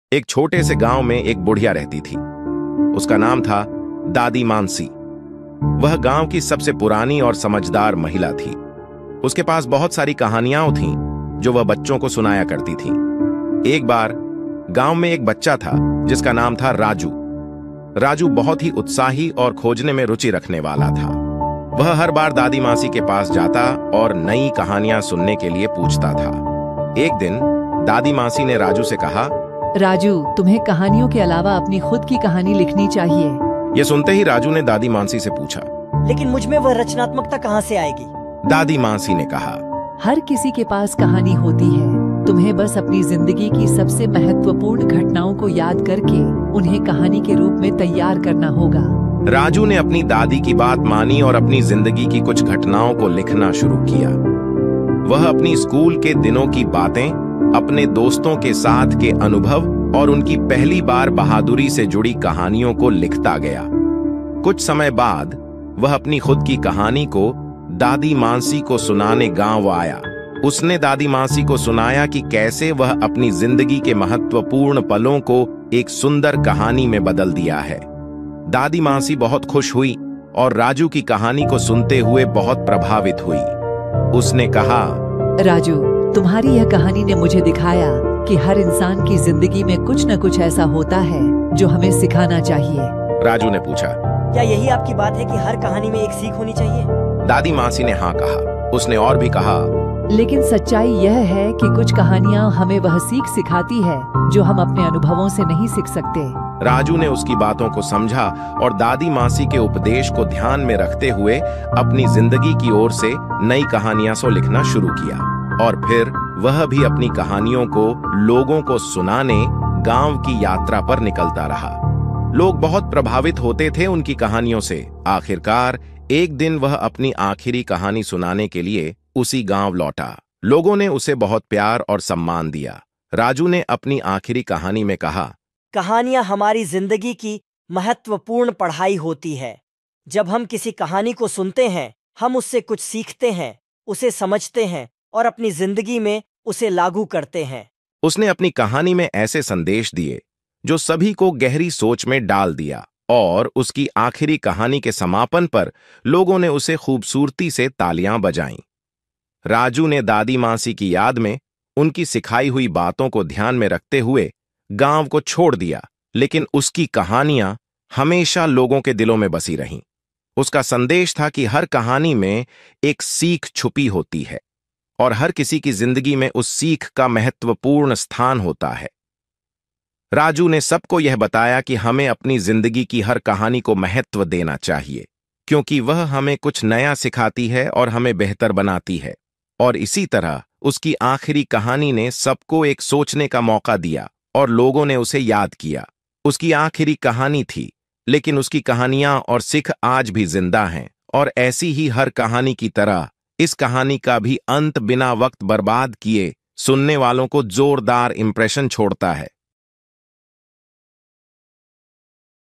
DADI MANSI KI KAHANIYAN : SHIKH OR JIBAN KI MAHEK.
Yea Hai ek jadugar ki kanhani. Jo Dadimansi or Raju ki Ruprekha me buni gayi hai.Ek gaon ki choti si duniya me is kanhani ne sikhaya ki har insaan ke jiban me kuch na kuch sikhne ki baatein chipi hoti hai.